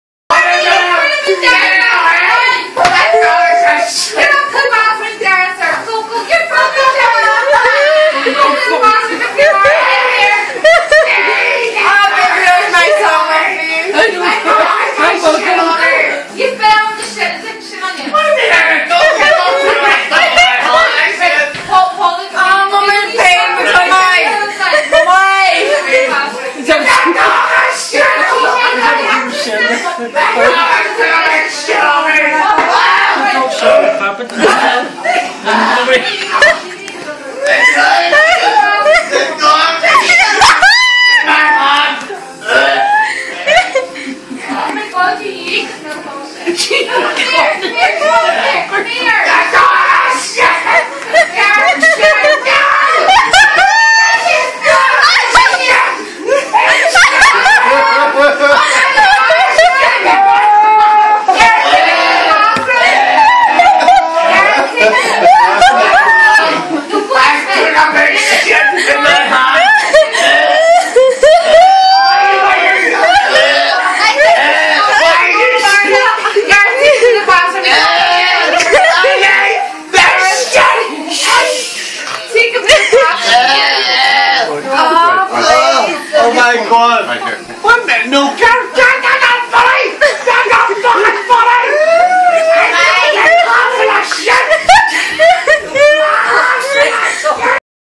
irish man puts hand in dog poo
My sister's boyfriend is drunk and accidently places his hand in dog poo, this is his reaction